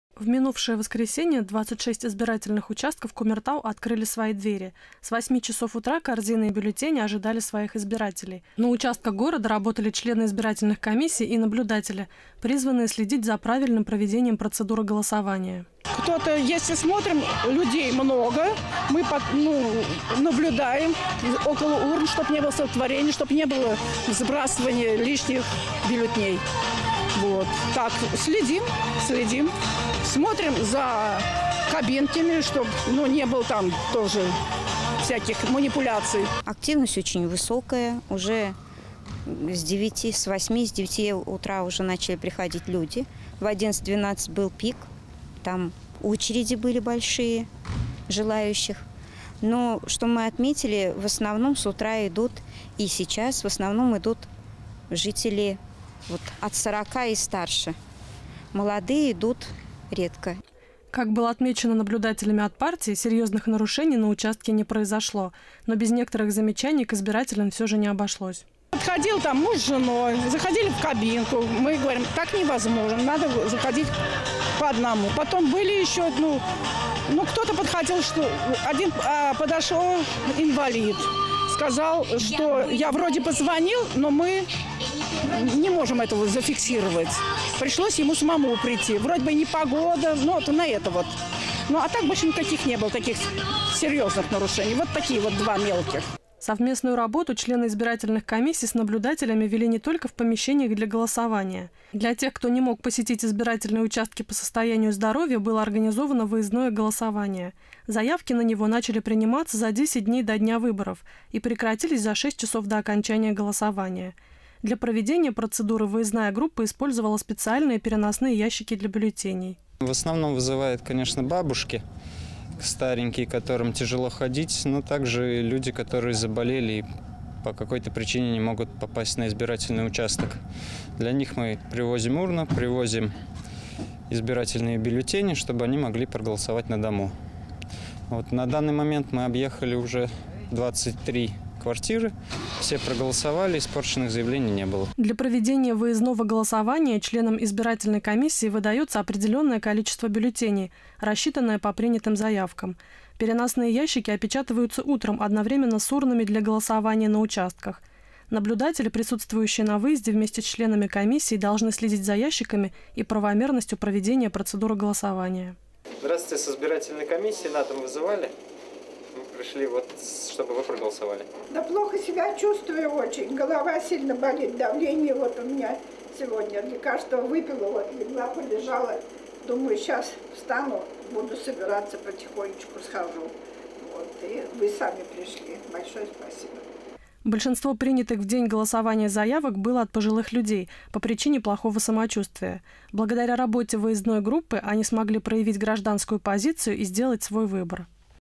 Выездное голосование
На каждом избирательном участке за процессом голосования следили наблюдатели от партий, чтобы выборы прошли честно и правомерно. Для тех, кто не смог посетить участки по состоянию здоровья были организованы голосования на дому.